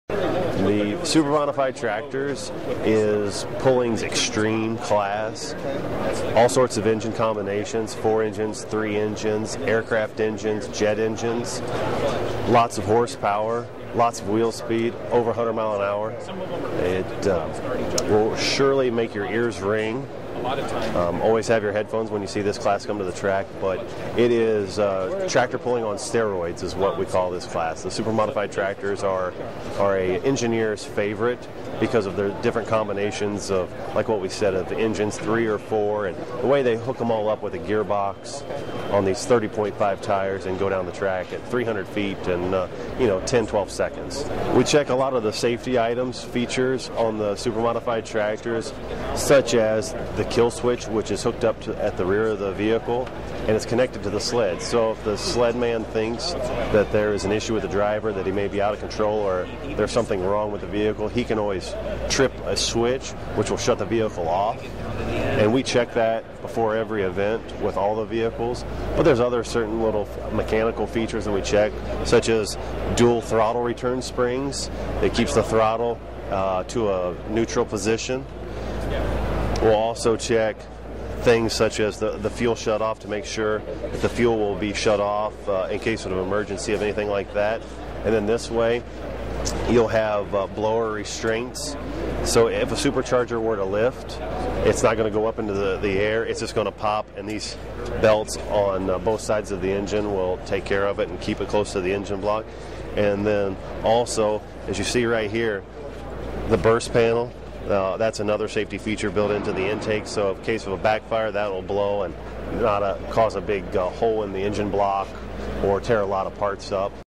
Tractor-Pull Inspectors Keep Sport Safe
Kill switches and other components get a review before National Championship Tractor Pull contestants can compete, says John Mears, tech official with the 2013 National Farm Machinery Show.